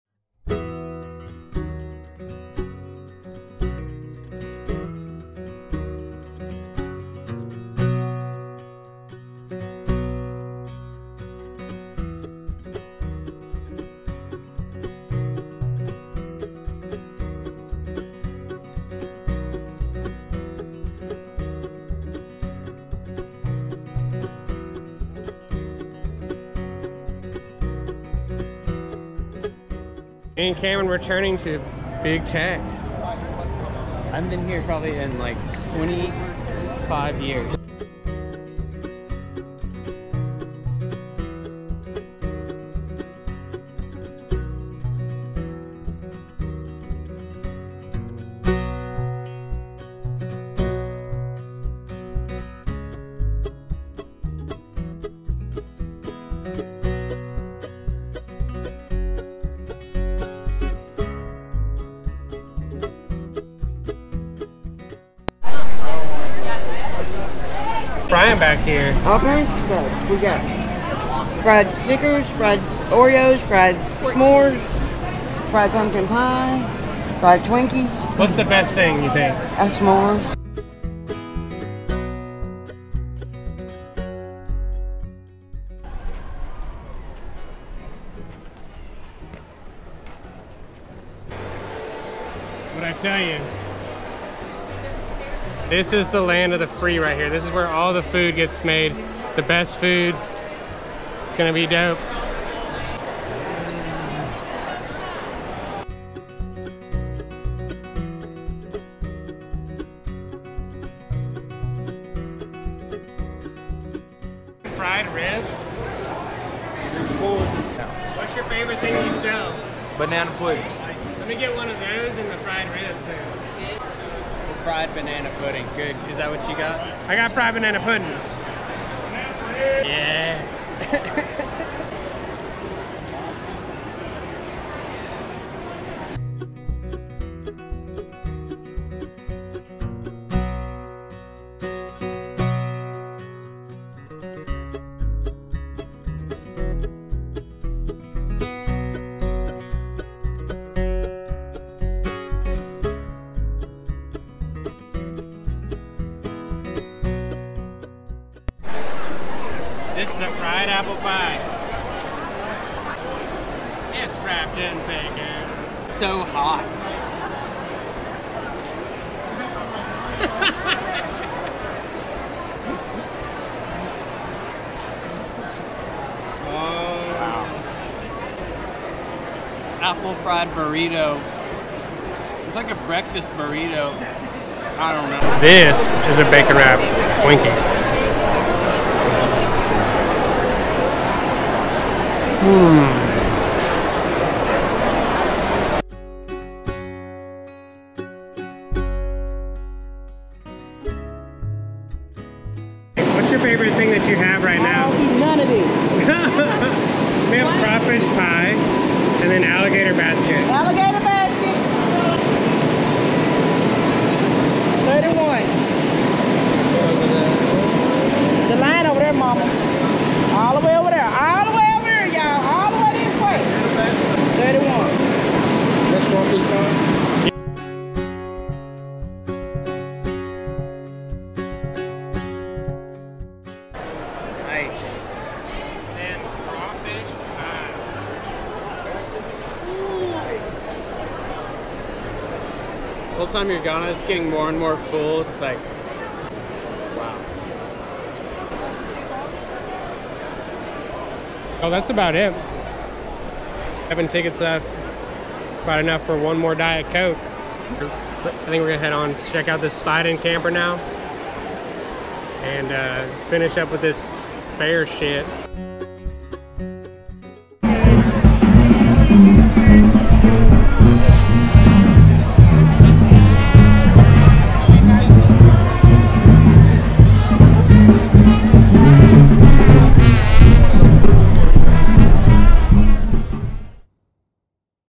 Short Trip to the Texas State Fair to Eat ALL THe THiNgS! Fair Tour for Rides, Cars, and Fried Foods
Giving me and my brother to catch up at the Texas state fair just like the old days